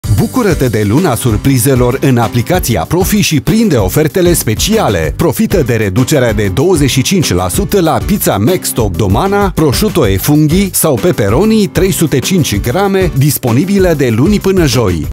Luna surprizelor continuă în aplicație!
O nouă zi, o nouă ofertă specială!
Doar de luni până joi ai în aplicație o reducere de 25% la pizza Maxtop Domana, prosciutto & funghi sau pepperoni, de 305g.